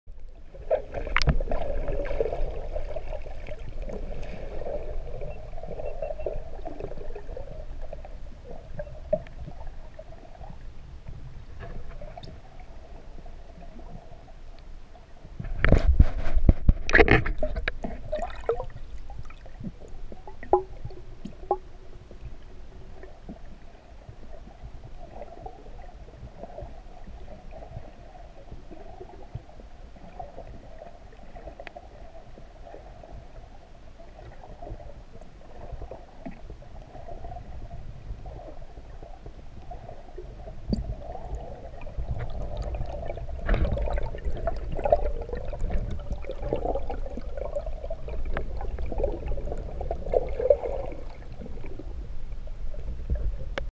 Free dive with Jelly Fish
Palau Jellyfish Lake Free Dive, (not a long breath hold I know)